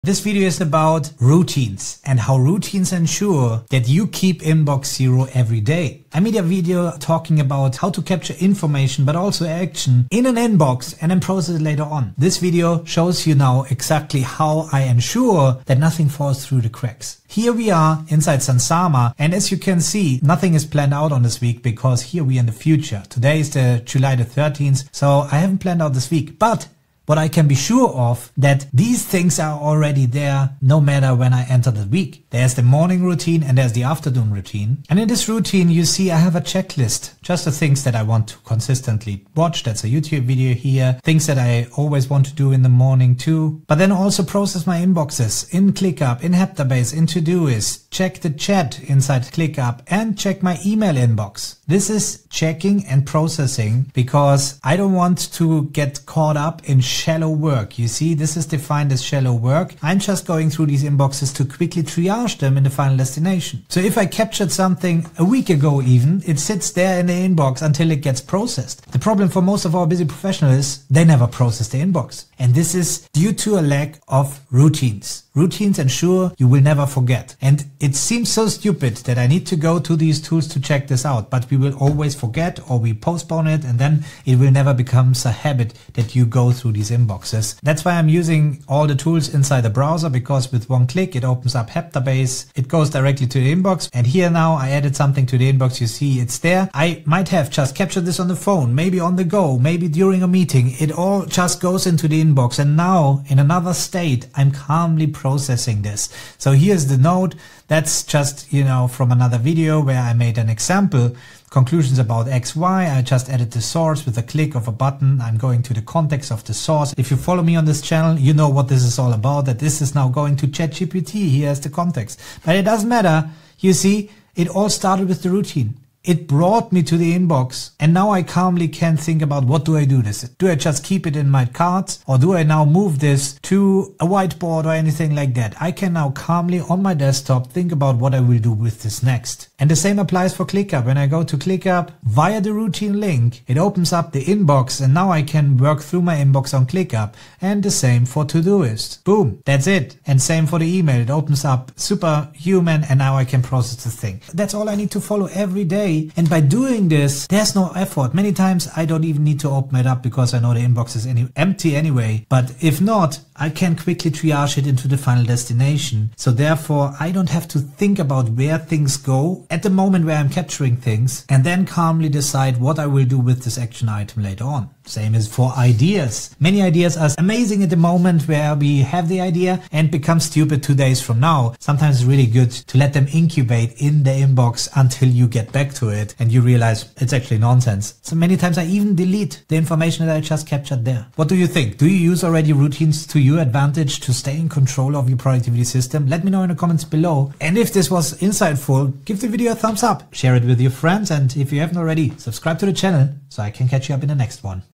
How I stay in control of all my tasks and notes
Keeping tasks and information organized, as well as maintaining an inbox of zero, is easier than you might think!